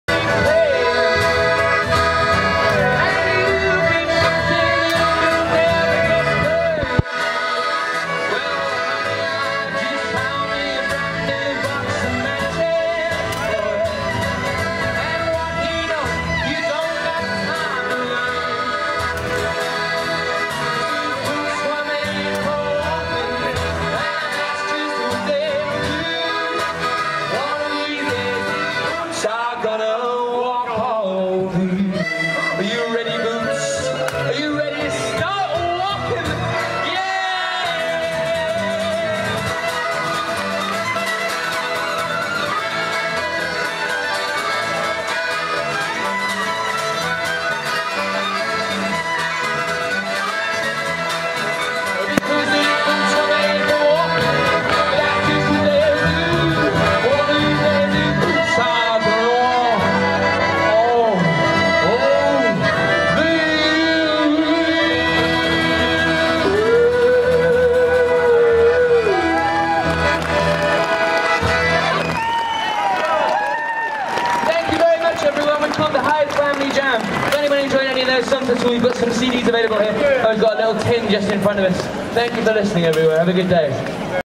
Hyde Family Jam
Hyde Family Jam busing in the centre of York